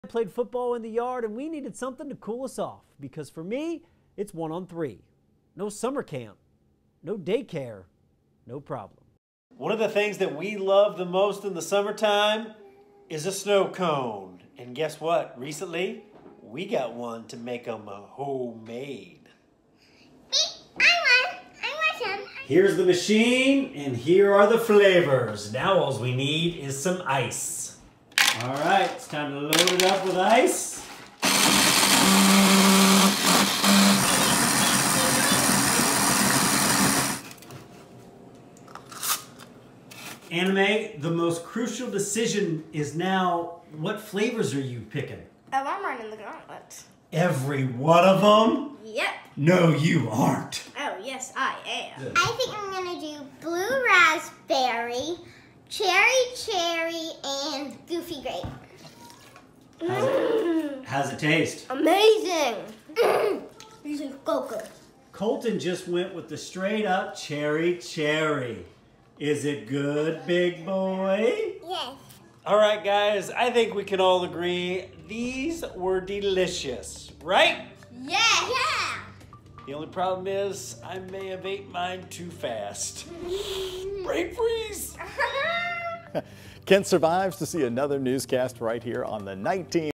Day 103: Homemade snow cones
Kent Spencer makes home made snow cones on Day 103 at home.